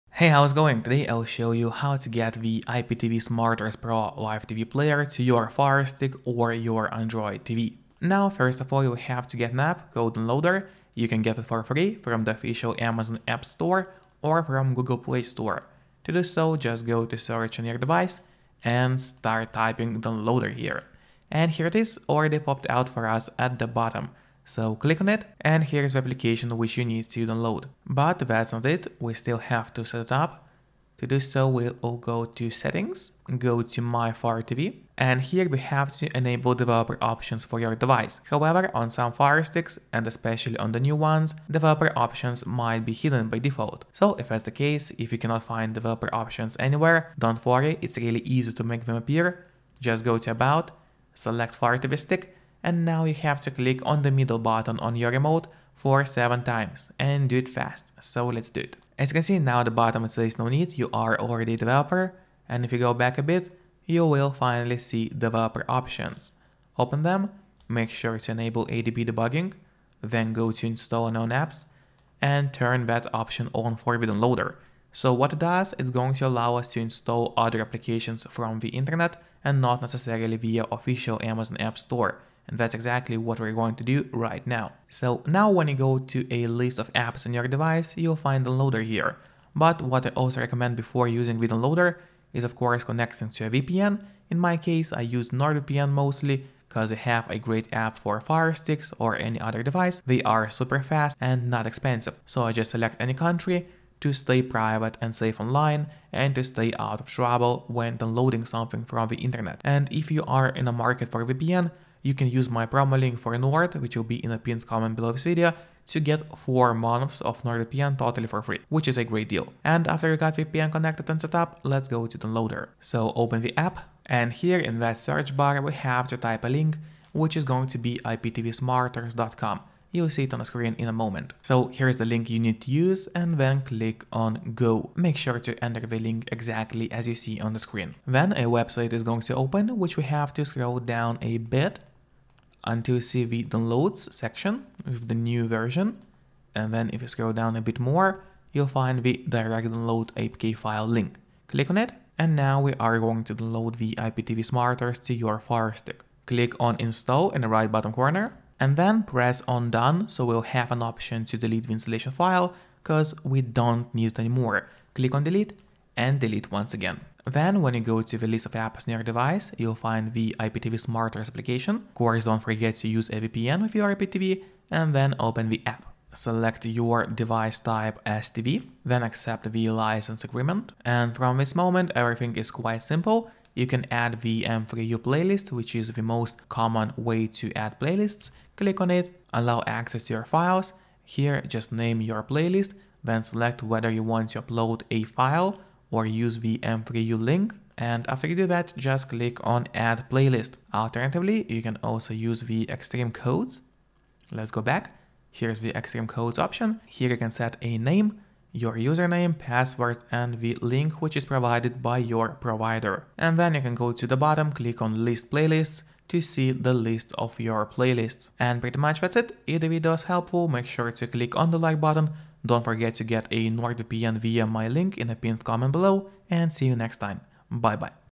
How to Download IPTV Smarters Pro Live TV Player to Firestick - Easy Guide
Wondering how to get the IPTV Smarters Pro app on your Firestick in 2024? Here's an easy tutorial on how to download and install the Smarters Pro live tv player on a Fire TV Stick device. Use this Smarters Pro tutorial to get the app on your Firestick.